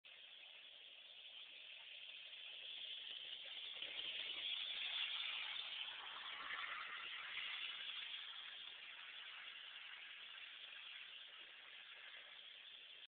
Fornicated Ruby in progress
The Fornicated Ruby in progress, both physically and  in construction